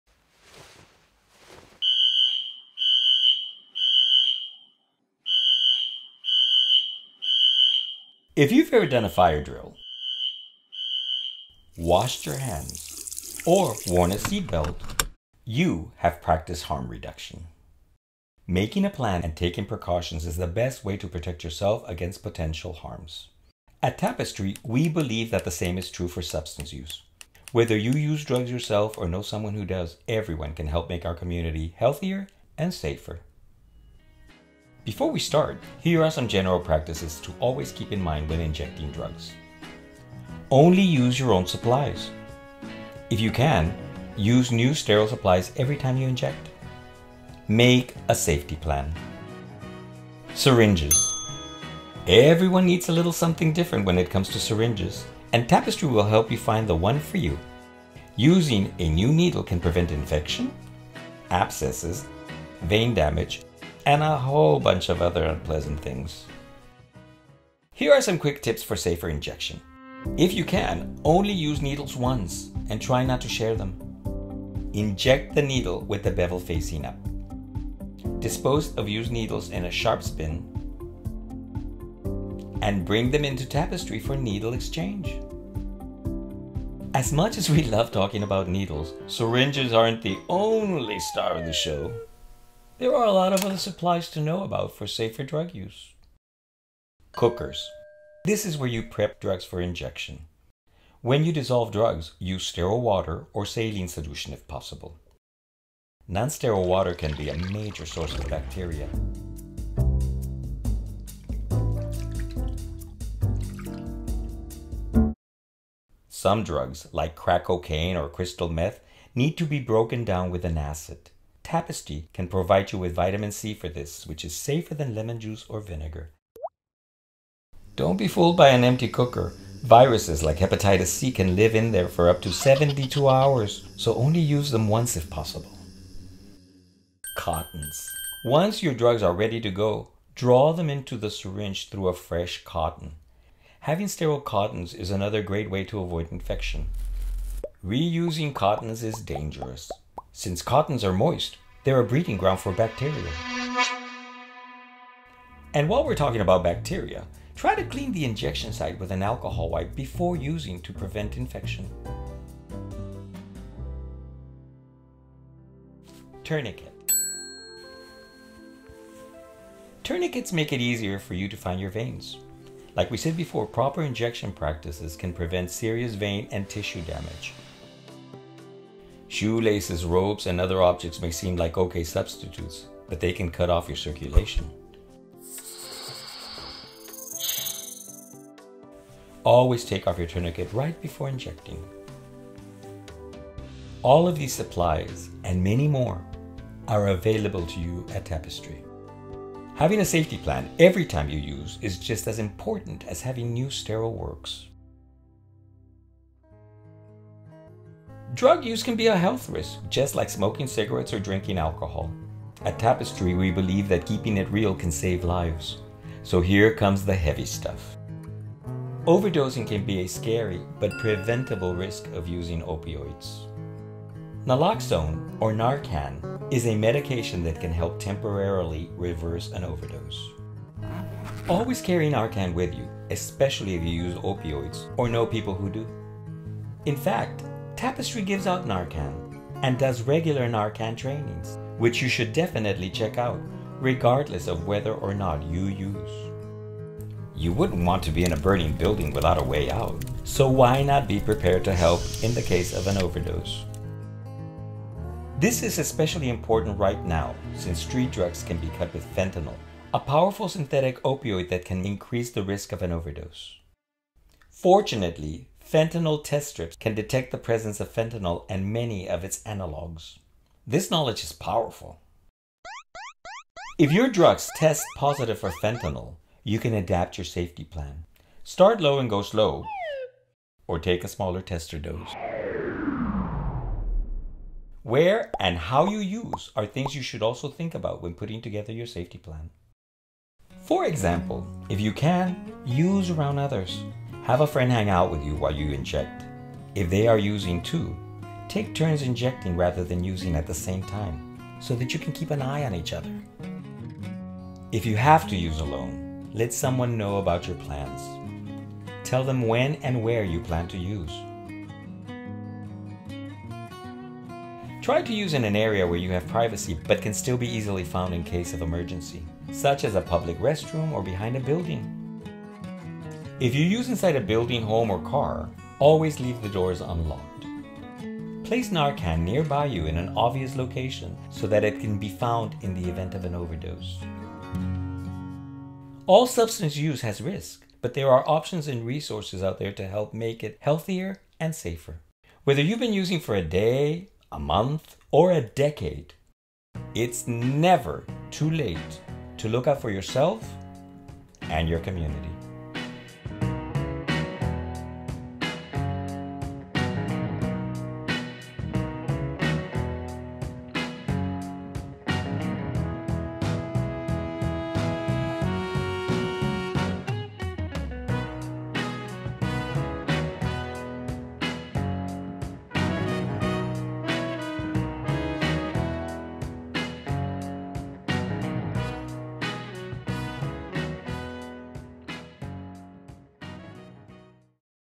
Safer Supplies Checklist - Tips for Safer Drug Use
This video reviews safer supplies and other resources available at Tapestry's Harm Reduction locations. Learn about supplies and tips for safer drug use to reduce the chance of fatal overdose, disease transmission, abscesses, infections, and other potential risks.